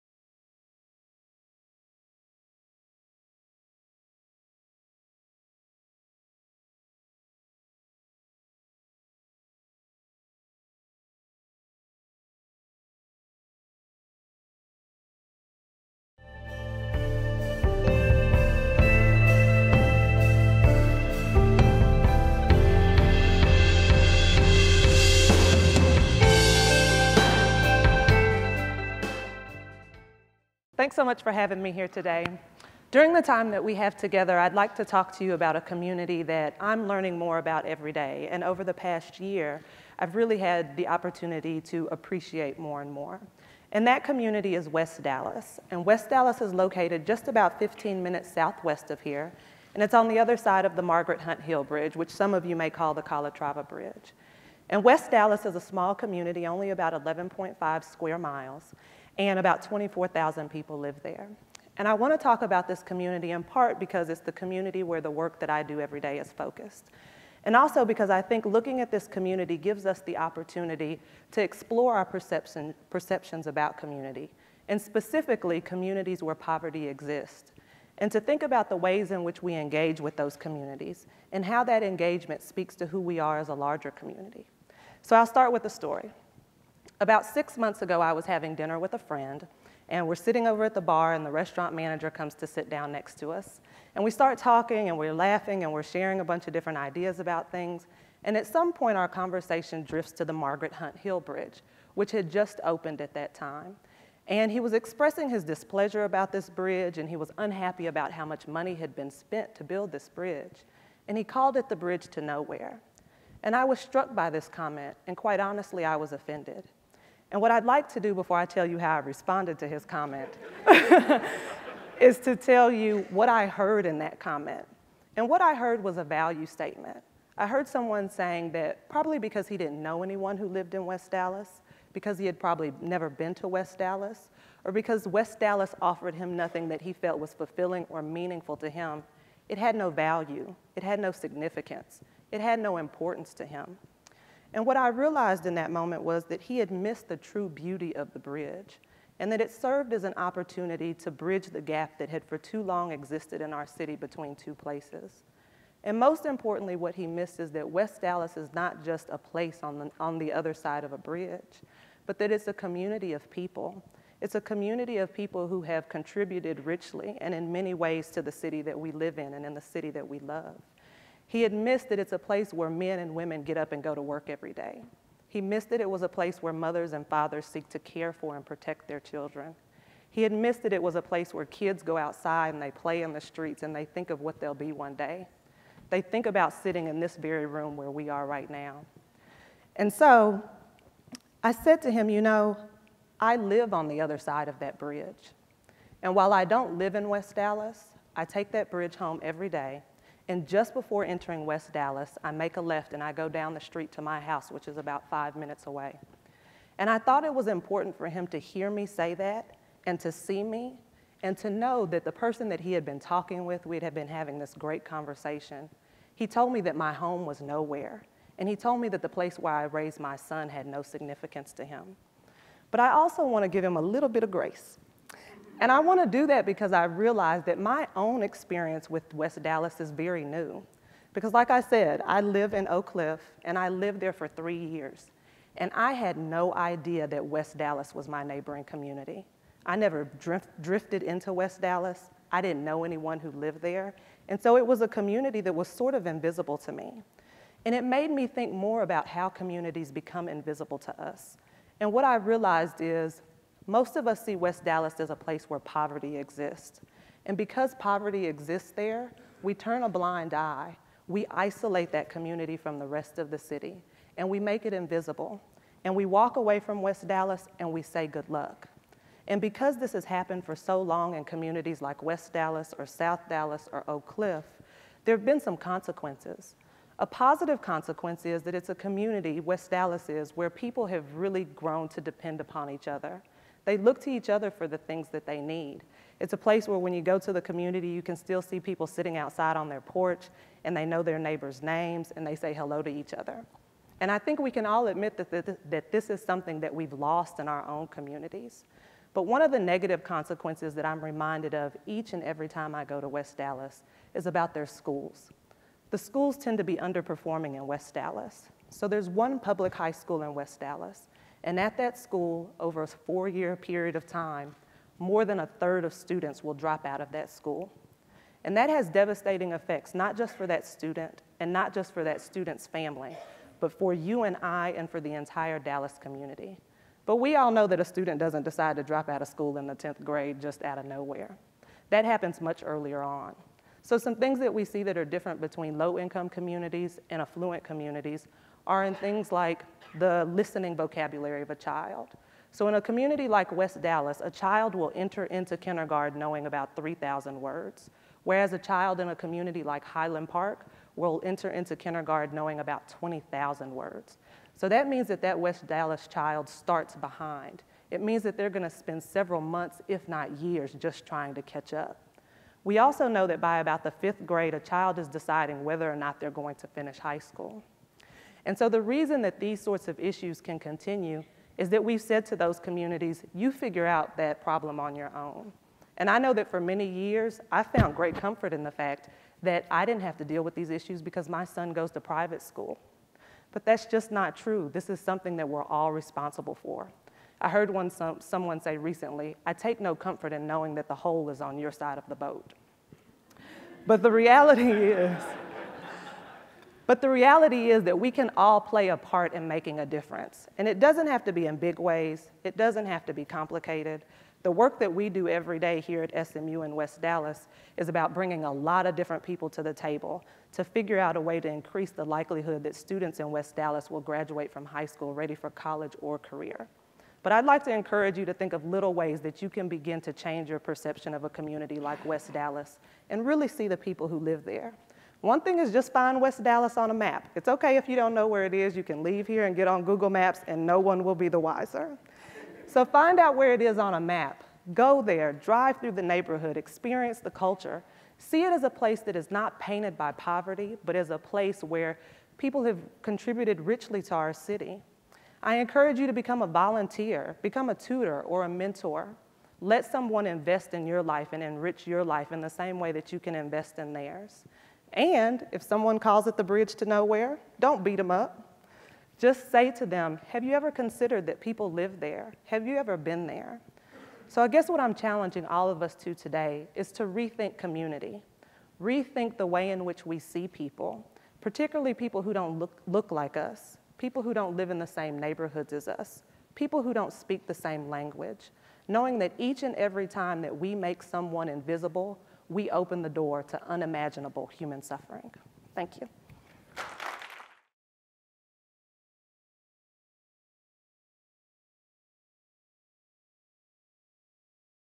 TEDxSMU Salon 2012 - Erin Crosby
Erin is the communications director for the Center on Communities and Education and also oversees student engagement, collaborating with SMU faculty and students to design civic engagement projects at schools and community organizations in The School Zone, West Dallas. She holds a Master's degree in print journalism from Boston University and a Bachelor's degree in English from SMU. In her talk, Erin will take us on a tour of West Dallas, the small, close-knit community at the foot of the so-called "Bridge to Nowhere" to explore perceptions about community, education transformation and collaborative partnerships.